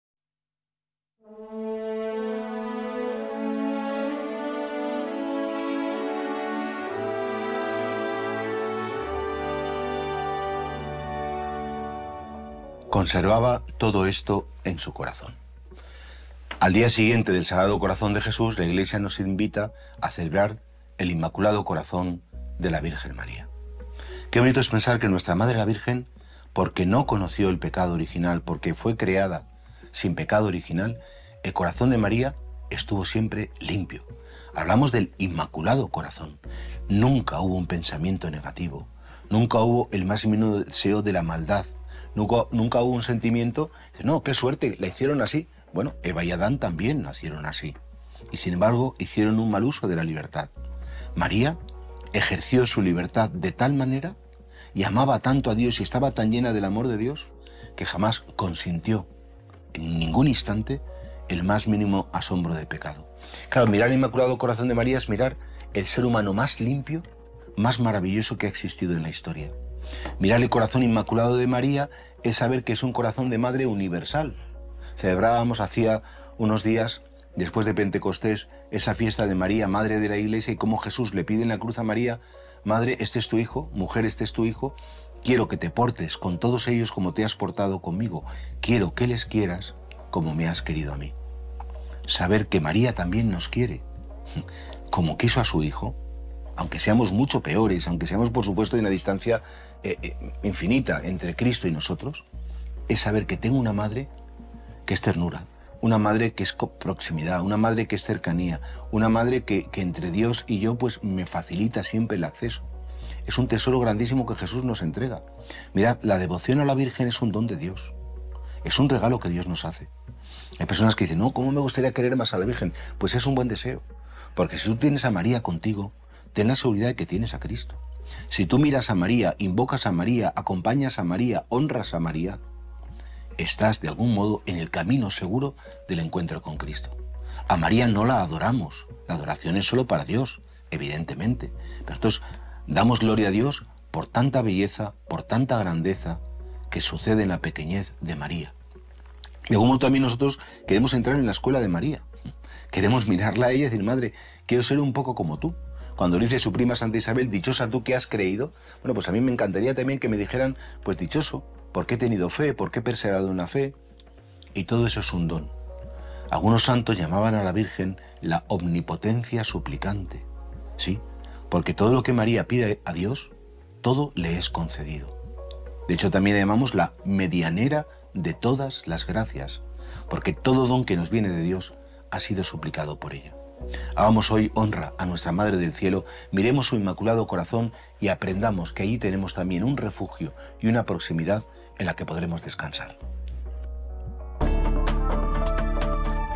08.06.2024 - Palabra de Vida «Conservaba todo esto en su corazón» Por P Jesús Higueras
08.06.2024 - Palabra de Vida «Conservaba todo esto en su corazón»   Por P  Jesús Higueras